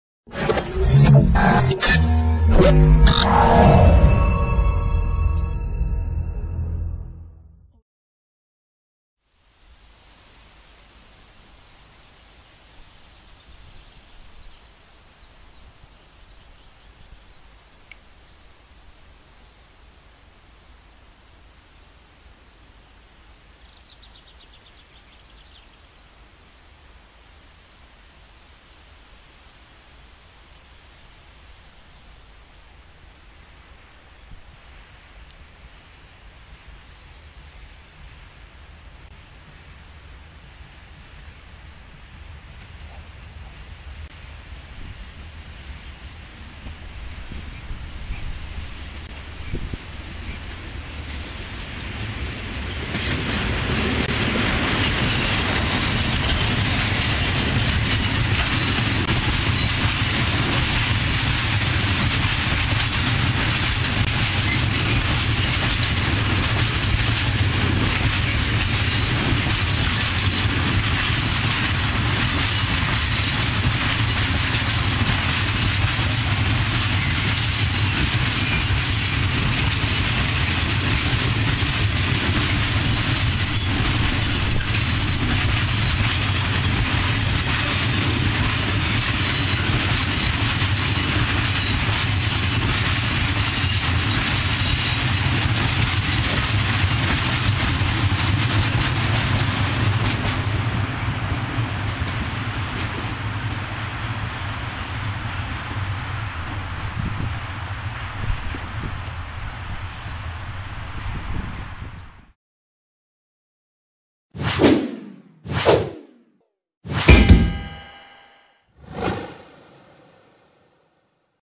[ PKP Cargo ] ET22-794 | TME 614031/0 Nowa Ruda Słupiec - Pruszków | Placencja 18/06/2015
PaFaWag 201E ET22-794
ze składem TME 614031/0 w relacji Nowa Ruda Słupiec - Pruszków kieruję się w stronę łącznicy Placencja - Bednary.